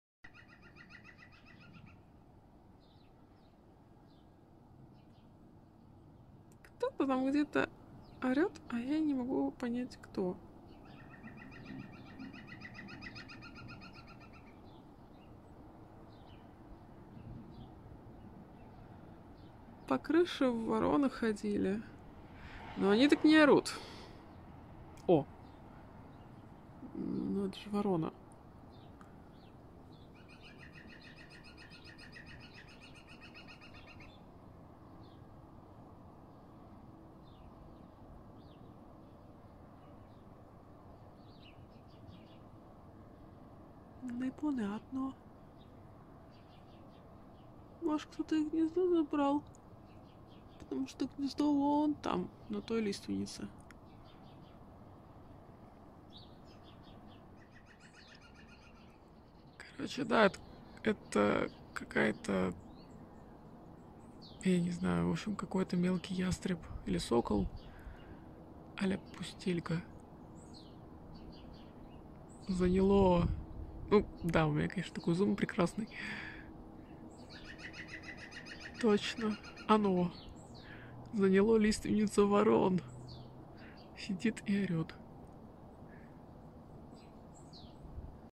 Скандалы, интриги, расследования! Кто же орёт на вороньей лиственнице?
Yesterday a bird tried claiming its territory on the crow's larch. I'm not sure what it is and the opinions divided between a kestrel and a northern goshawk 🤔

Мнения разделились: от пустельги до тетеревятника, врана или даже дятла о.О  😉